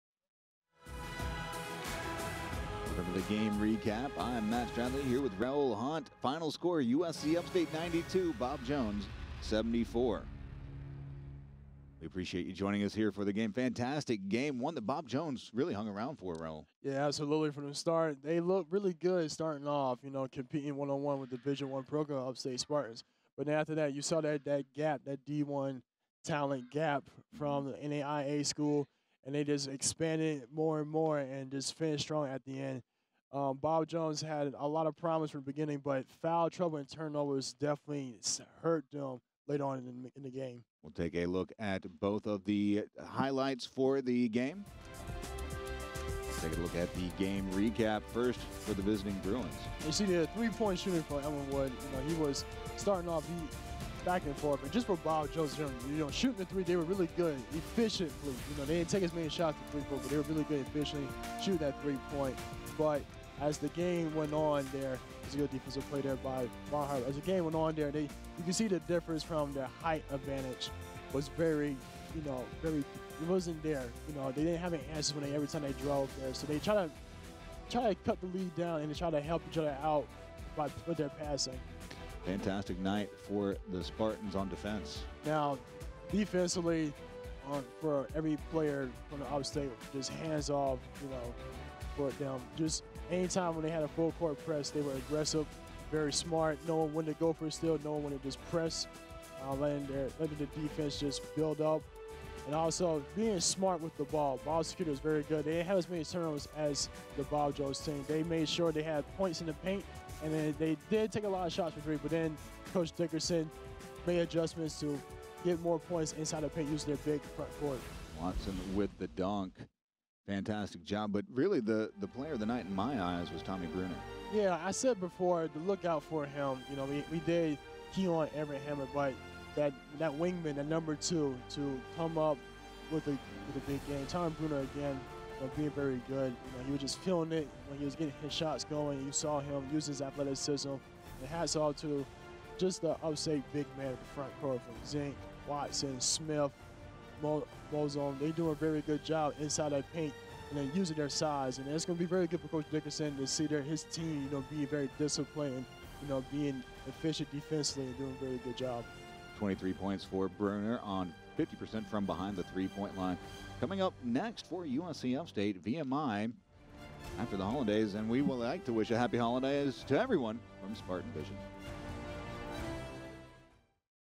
USC Upstate vs Bob Jones Recap 12.21.19
Here is a quick recap of today's victory vs Bob Jones University.